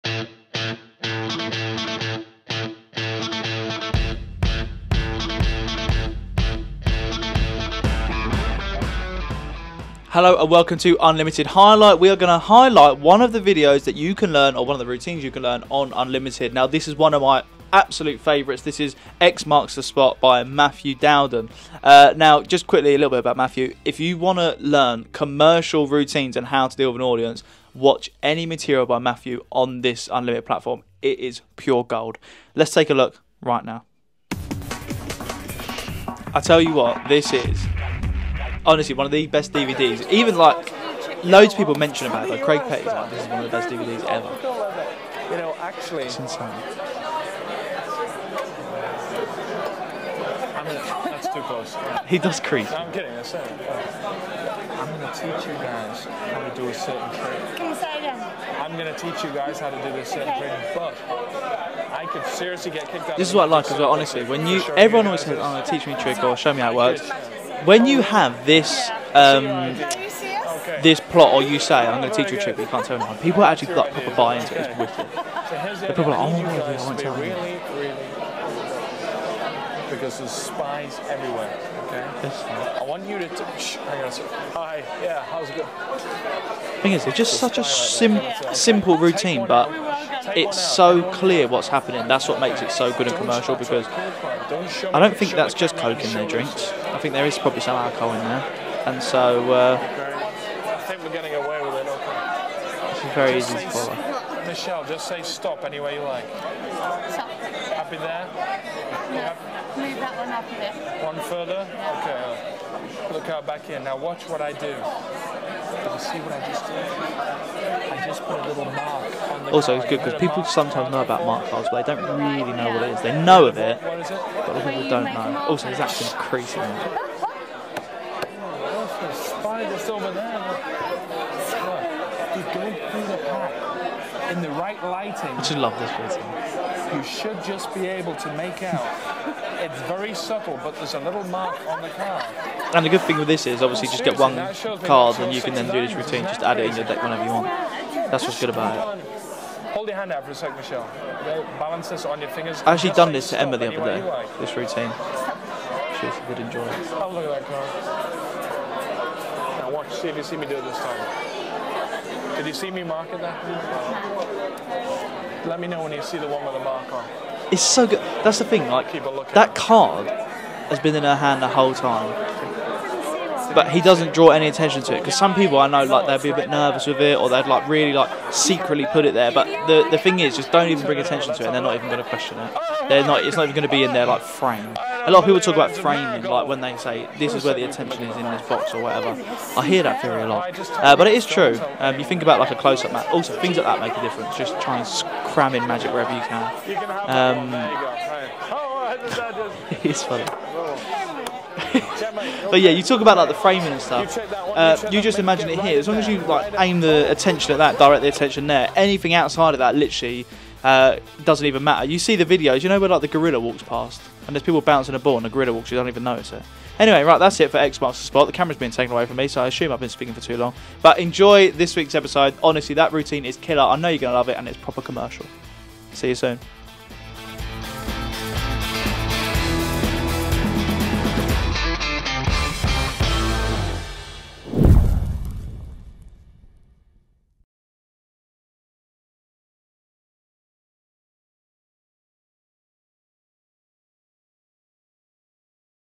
Unlimited Highlight ft X Marks The Spot by Matthew Dowden
In this killer, commercial routine, Matthew lets his audience in behind the scenes before blowing their minds!